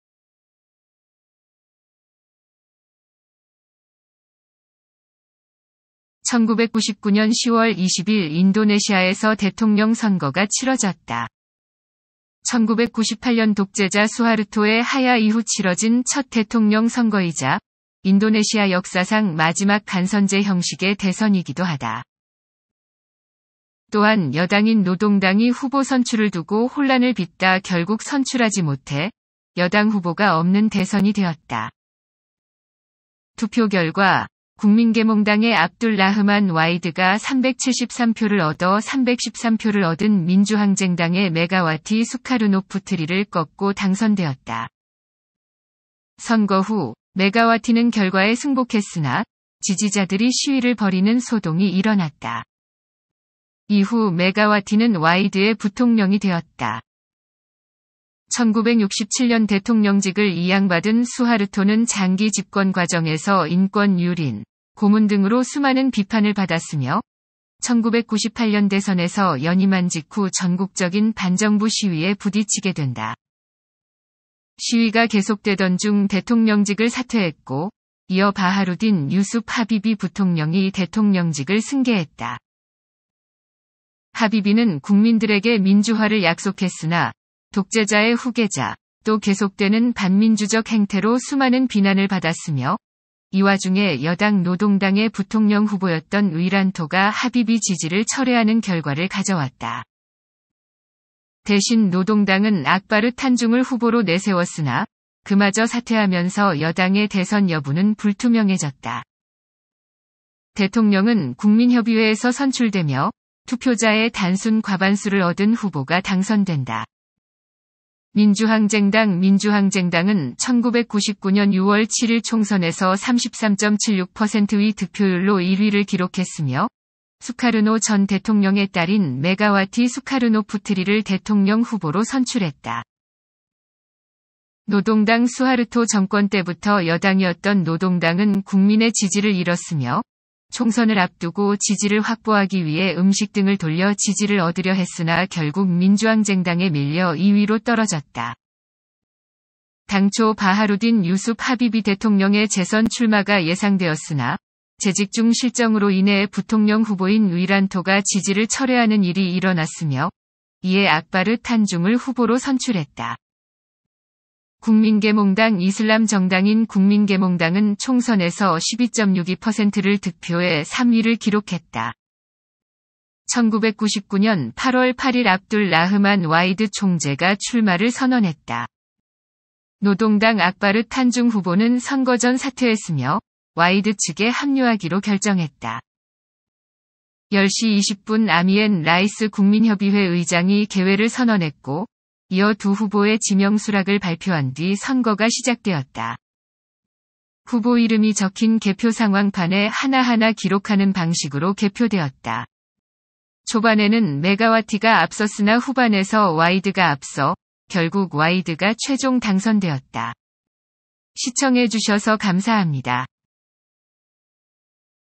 1999년 인도네시아 대통령 선거
1999년 10월 20일 인도네시아에서 대통령 선거가 치러졌다.
 1998년 독재자 수하르토의 하야 이후 치러진 첫 대통령 선거이자, 인도네시아 역사상 마지막 간선제 형식의 대선이기도 하다.
 또한 여당인 노동당이 후보 선출을 두고 혼란을 빚다 결국 선출하지 못해, 여당 후보가 없는 대선이 되었다.
 투표 결과, 국민계몽당의 압둘 라흐만 와히드가 373표를 얻어 313표를 얻은 민주항쟁당의 메가와티 수카르노푸트리를 꺾고 당선되었다.
 선거 후, 메가와티는 결과에 승복했으나, 지지자들이 시위를 벌이는 소동이 일어났다.
 이후 메가와티는 와히드의 부통령이 되었다.
 1967년 대통령직을 이양받은 수하르토는 장기 집권 과정에서 인권 유린, 고문 등으로 수많은 비판을 받았으며, 1998년 대선에서 연임한 직후 전국적인 반정부 시위에 부딪히게 된다.
 시위가 계속되던 중 대통령직을 사퇴했고, 이어 바하루딘 유숩 하비비 부통령이 대통령직을 승계했다.
 하비비는 국민들에게 민주화를 약속했으나, 독재자의 후계자, 또 계속되는 반민주적 행태로 수많은 비난을 받았으며, 이 와중에 여당 노동당의 부통령 후보였던 위란토가 하비비 지지를 철회하는 결과를 가져왔다.
 대신 노동당은 악바르 탄중을 후보로 내세웠으나, 그마저 사퇴하면서 여당의 대선 여부는 불투명해졌다.
 (해당 부분은 1999년을 기준으로 한 것이며, 현재는 개정되었음) 대통령은 국민협의회(MPR)에서 선출되며, 투표자의 단순 과반수를 얻은 후보가 당선된다.
 민주항쟁당 민주항쟁당은 1999년 6월 7일 총선에서 33.76%의 득표율로 1위를 기록했으며, 수카르노 전 대통령의 딸인 메가와티 수카르노푸트리를 대통령 후보로 선출했다.
 노동당(골카르) 수하르토 정권 때부터 여당이었던 노동당은 국민의 지지를 잃었으며, 총선을 앞두고 지지를 확보하기 위해 음식 등을 돌려 지지를 얻으려 했으나 결국 민주항쟁당에 밀려 2위로 떨어졌다.
 당초 바하루딘 유숩 하비비 대통령의 재선 출마가 예상되었으나, 재직 중 실정으로 인해 부통령 후보인 위란토가 지지를 철회하는 일이 일어났으며, 이에 악바르 탄중을 후보로 선출했다.
 국민계몽당 이슬람 정당인 국민계몽당은 총선에서 12.62%를 득표해 3위를 기록했다.
 1999년 8월 8일 압둘 라흐만 와히드 총재가 출마를 선언했다.
 노동당 악바르 탄중 후보는 선거 전 사퇴했으며, 와히드 측에 합류하기로 결정했다.
 10시 20분 아미엔 라이스 국민협의회 의장이 개회를 선언했고, 이어 두 후보의 지명 수락을 발표한 뒤 선거가 시작되었다.
 후보 이름이 적힌 개표상황판에 하나하나 기록하는 방식으로 개표되었다.
 초반에는 메가와티가 앞섰으나 후반에서 와히드가 앞서, 결국 와히드가 최종 당선되었다.